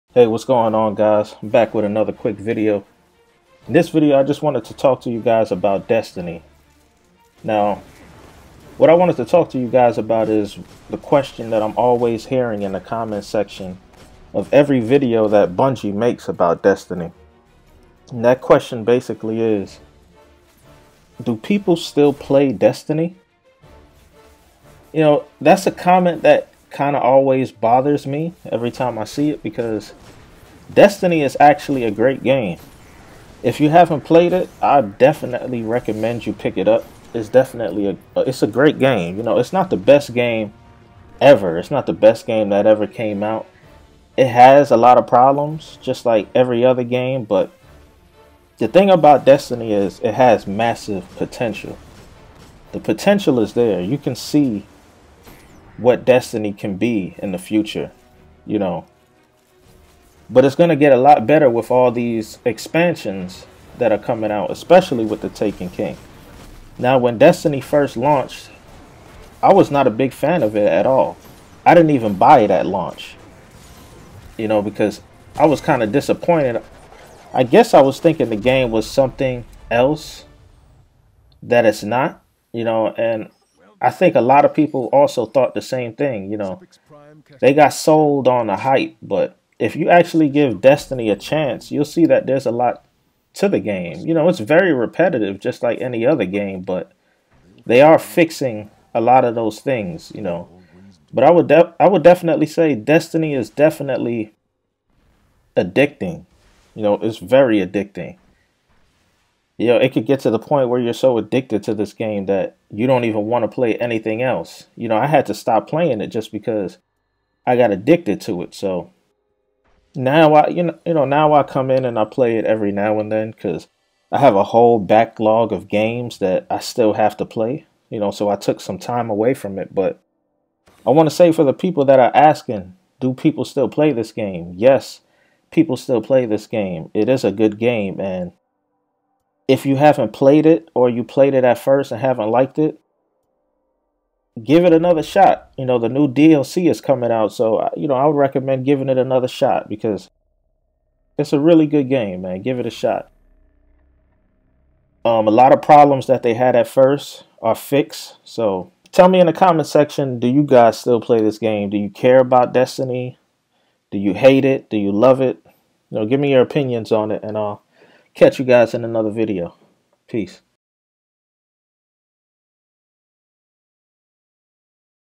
Do People Play Destiny?
In this video I discuss whether or not people still play Destiny. The one question people always ask. Do people still play Destiny?